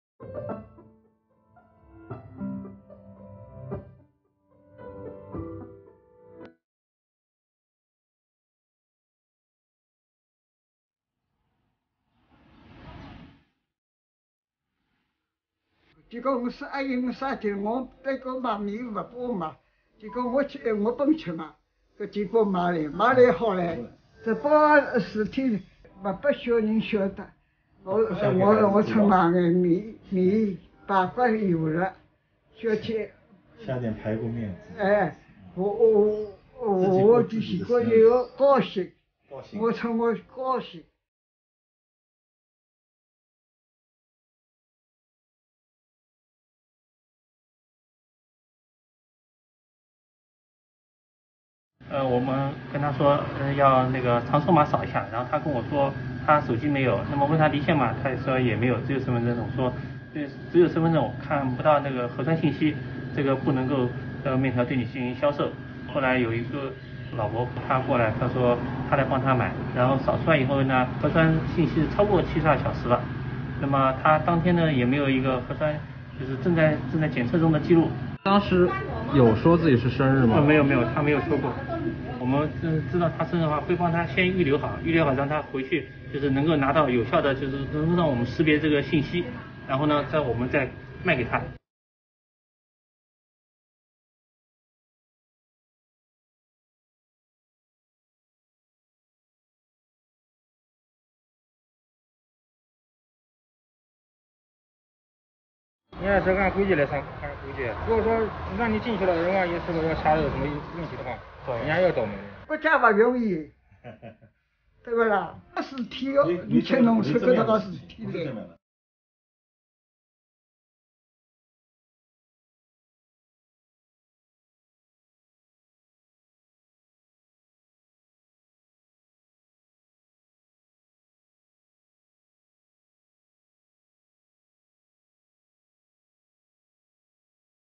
上海一老人生日当天因无离线码无法买面？当事阿婆作出回应
6月18日7时许，上海一老人因未带手机无法扫描场所码，而未能在生日当天于第六粮油愚园路店购买切面引发网络热议。6月21日，该门店及当事人阿婆对此事作出回应。